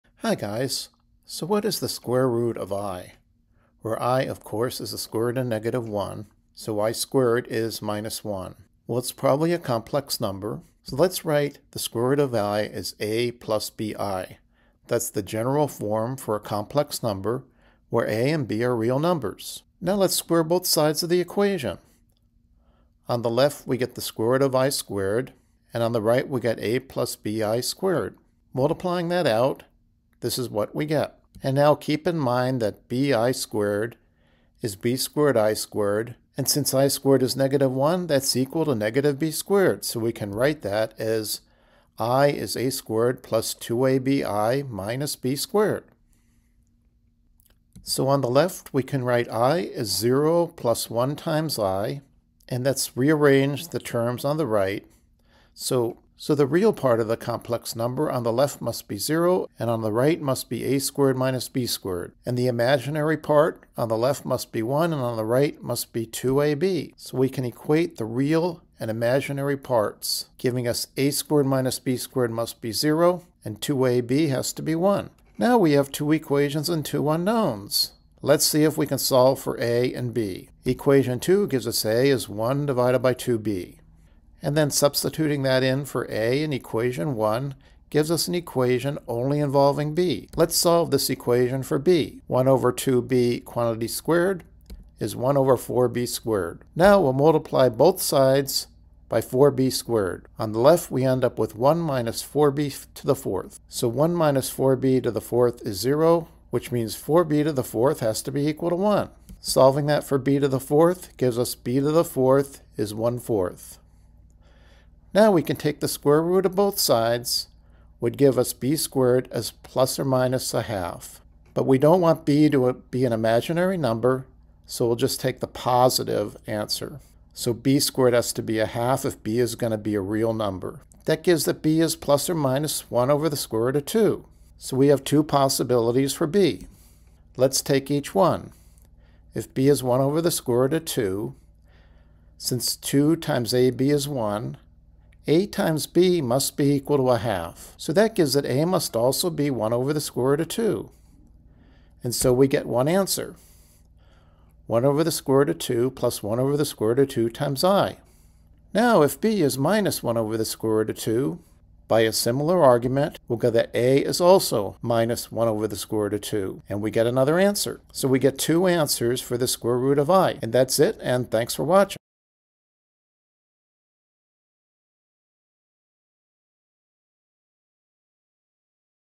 What is the square root of i
Can you find the square root of i?

In this video we use basic properties of complex numbers to find the square root of i.  We solve two equations in two unknowns using a bit of Algebra.  All Math steps are explained in detail.